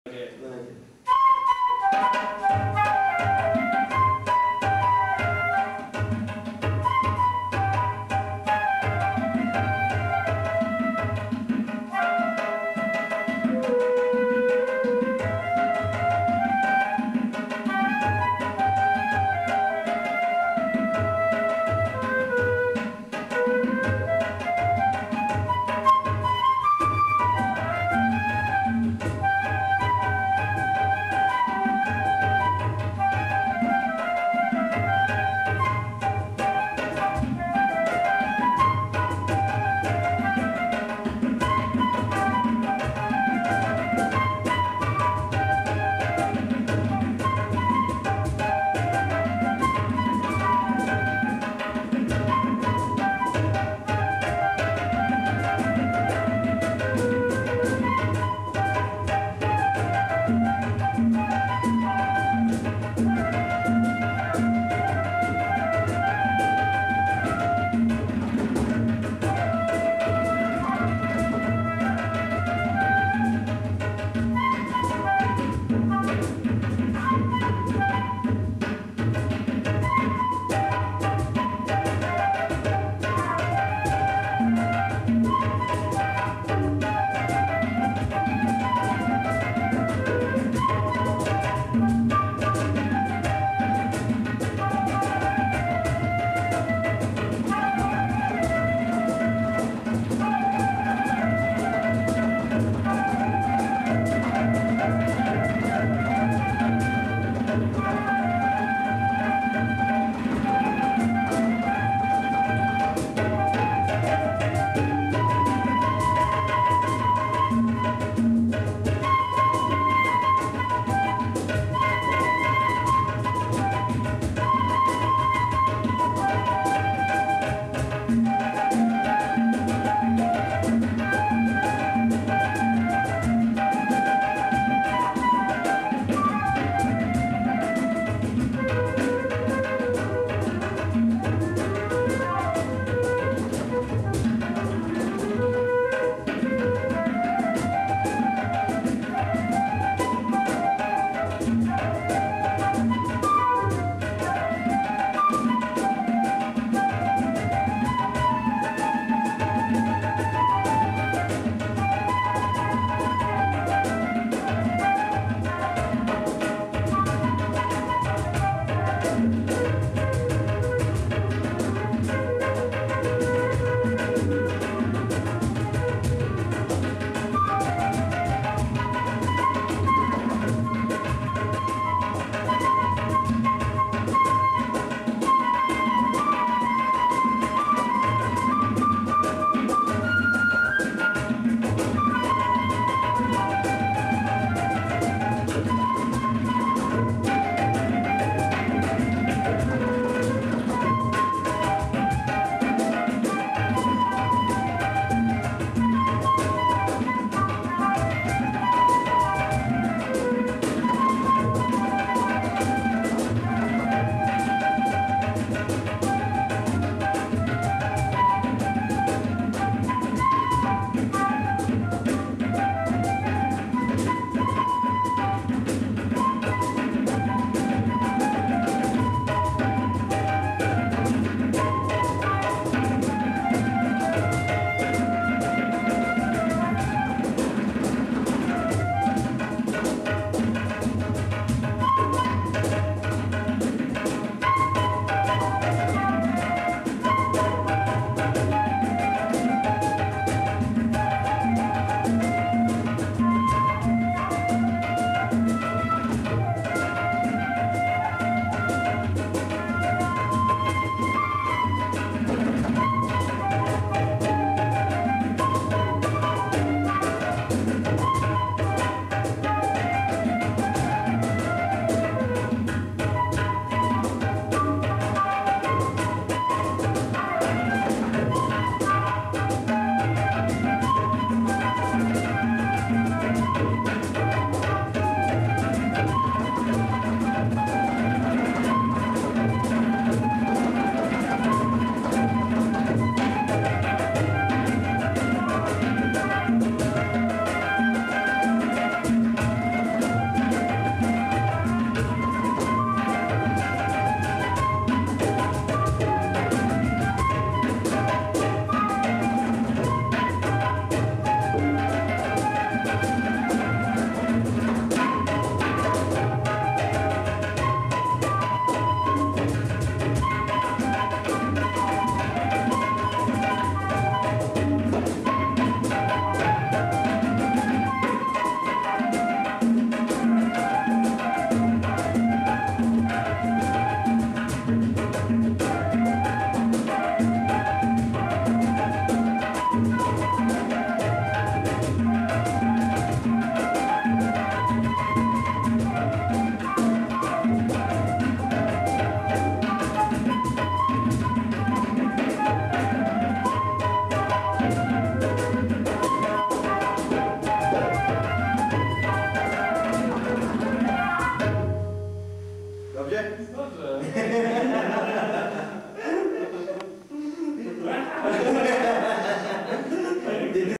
Flute Tabla Cajon
Flute - Martin, Tabla - Anupam, Cajon - Lukas